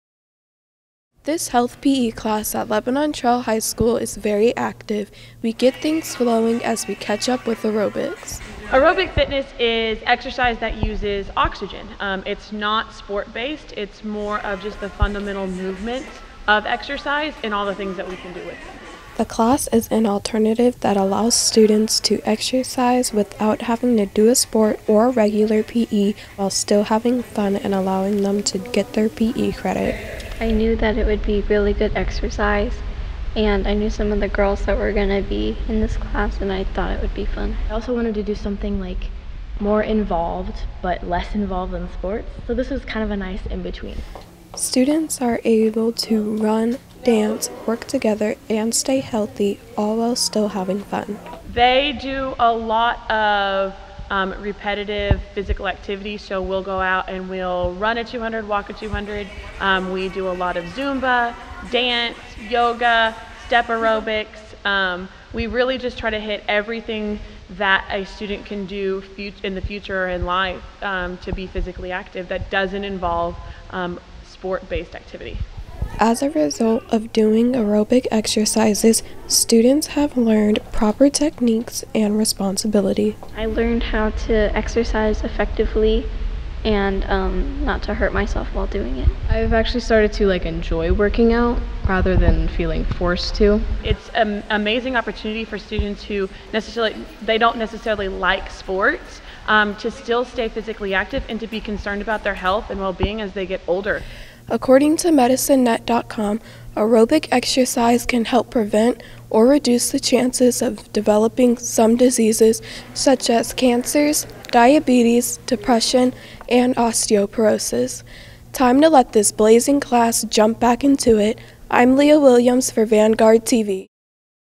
The Aerobics Alternative
VanguardTV's Leah Williams gets in step with the LTHS Aerobics class that serves as an alternative to traditional physical education.